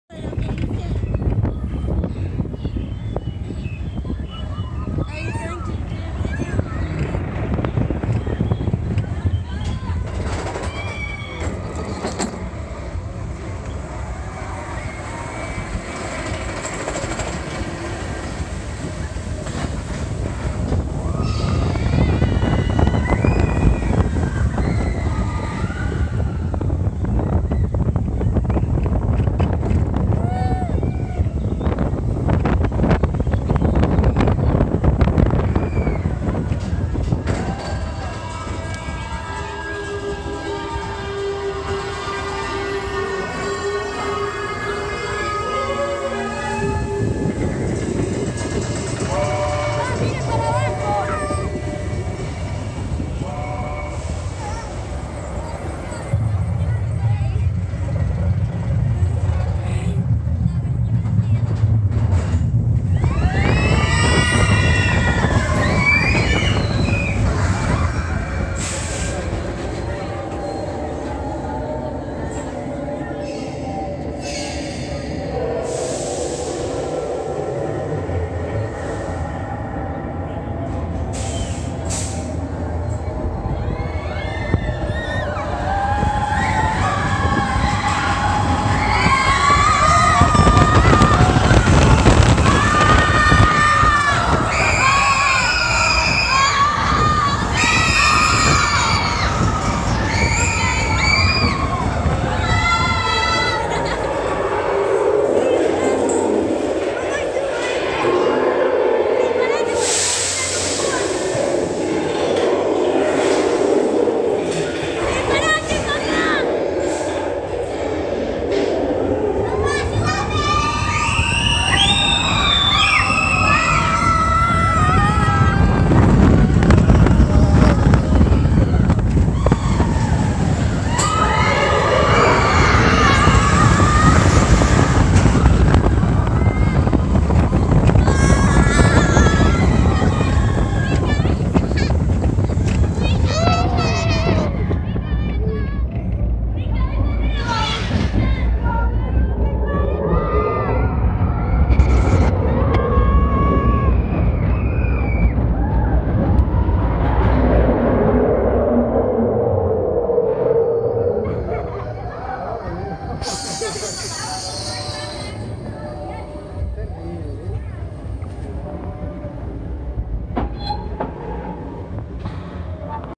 Expedition Everest Full Front Row POV 2017 Disney's Animal Kingdom
Journey up Mount Everest and escape a Yeti in the most expensive roller coaster ever built.